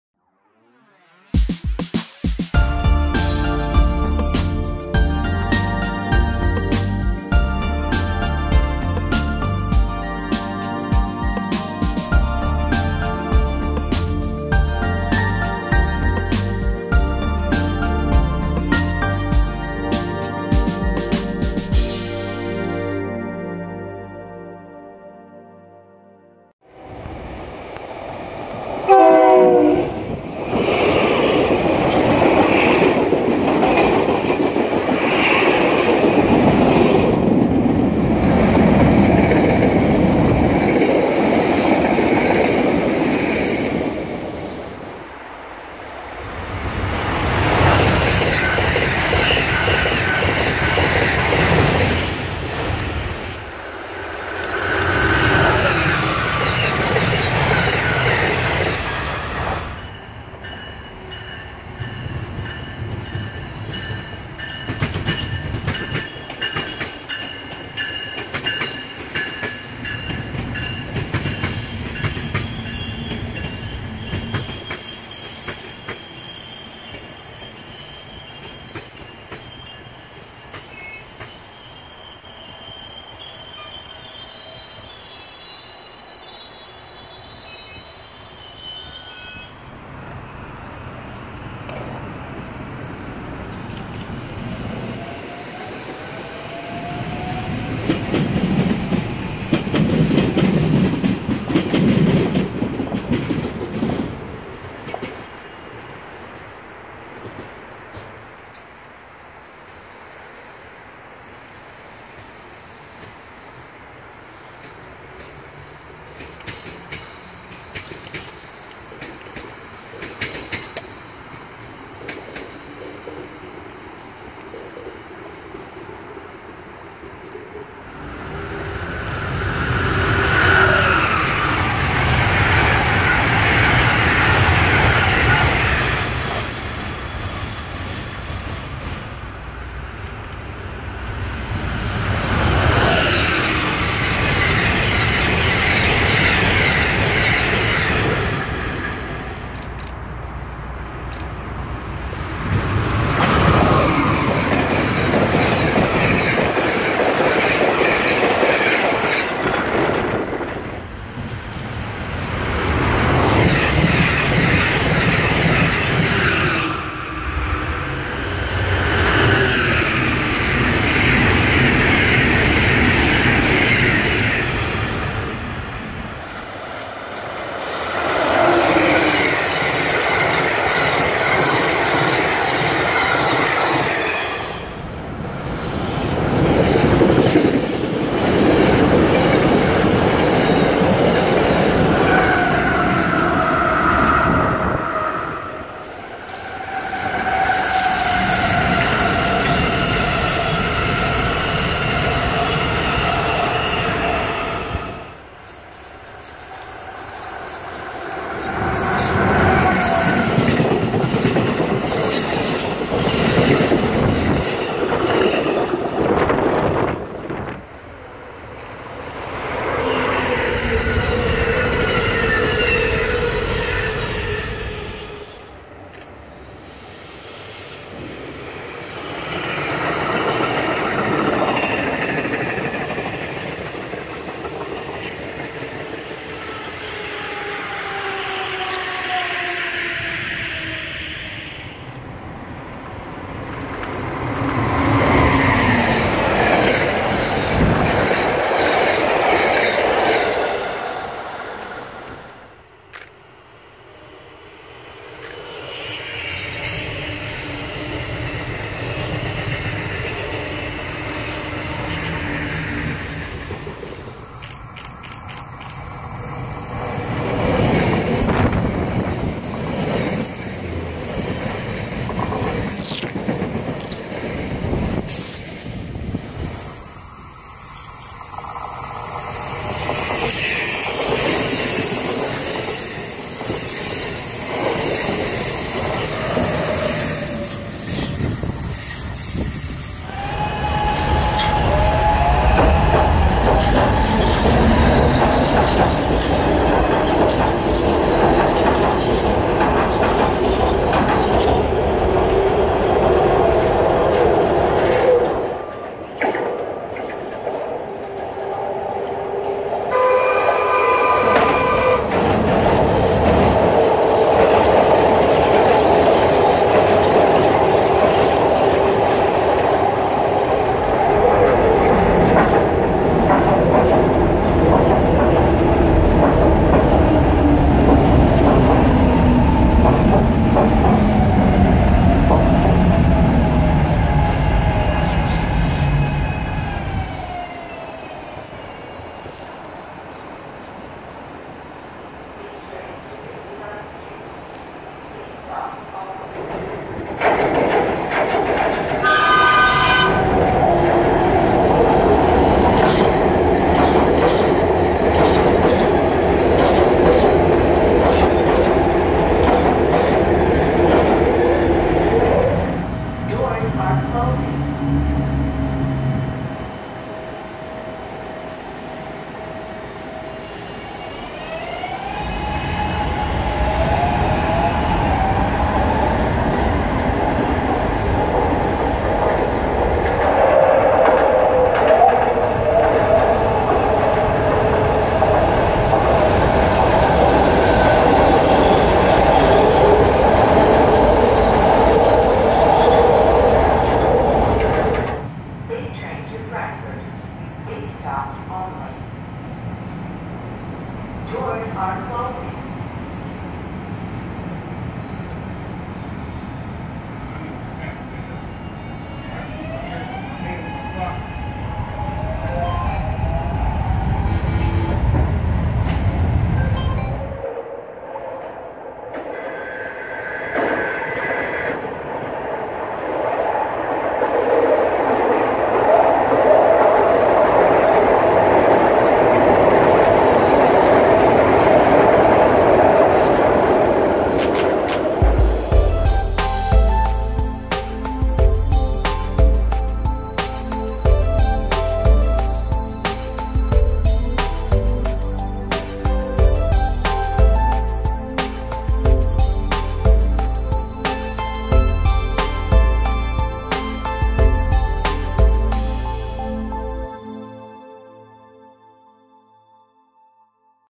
2020 Choo Choos Episode 4
Production has been down due to personal tech issues, but I'm back and ready to share some previously unreleased work. Here's a video from February-early March 2020. Includes the last clips of SEPTA's Market Frankford Line A/B skip stop service before permanent discontinuation (Somerset and Berks Stations). Railroad clips include North Broad, Holmesburg Jct, Overbrook, Villanova, Strafford, and Berwyn.